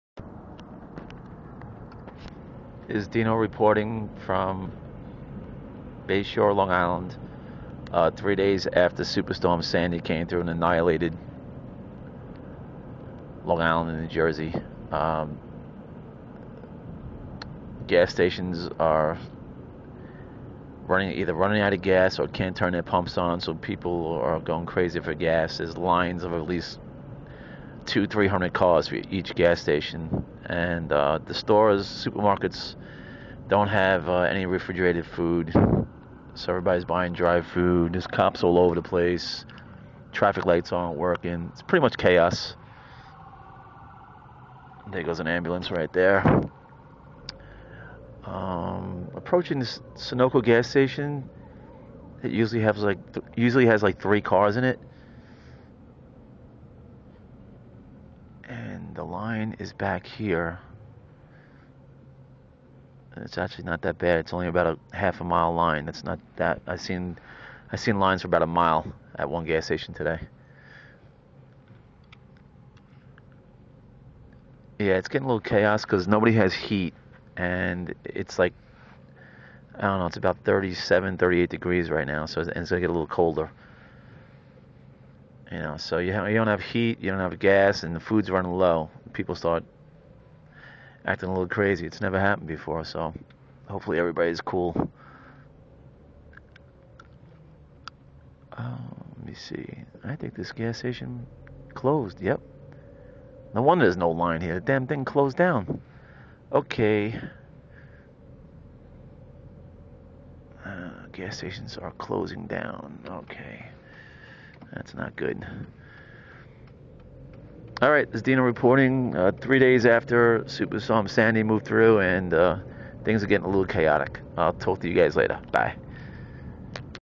Dino describes the chaos after Superstorm Sandy ...
Location:  Long Island